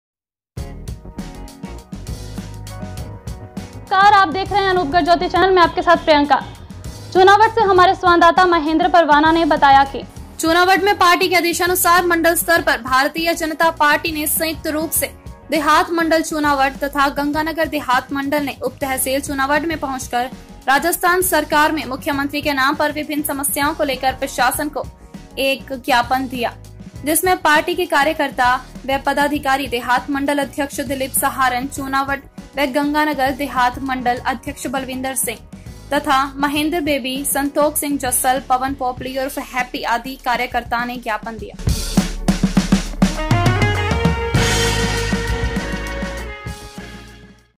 चूनावढ़: राजस्थान सरकार में मुख्यमंत्री के नाम विभिन्न समस्याओं को लेकर प्रशासन को एक ज्ञापन दिया
चूनावढ़ मे पार्टी के आदेशानुसार मंडल स्तर पर भारतीय जनता पार्टी ने संयुक्त रूप से देहात मंडल चुनावढ व गंगानगर देहात मंडल ने उप तहसील चुनावढ में पहुंचकर राजस्थान सरकार में मुख्यमंत्री के नाम विभिन्न समस्याओं को लेकर प्रशासन को एक ज्ञापन दिया गया जिसमें पार्टी के कार्यकर्ता वह पदाधिकारी देहात मंडल अध्यक्ष  दलीप सहारण चुनावढ वह गंगा नगर देहात मंडल अध्यक्ष बलविंदर सिंह वह  महेंद्र बेबी संतोख सिंह जस्सल पवन पॉपली उर्फ हैप्पी आदि कार्यकर्ता ने ज्ञापन सौंपा।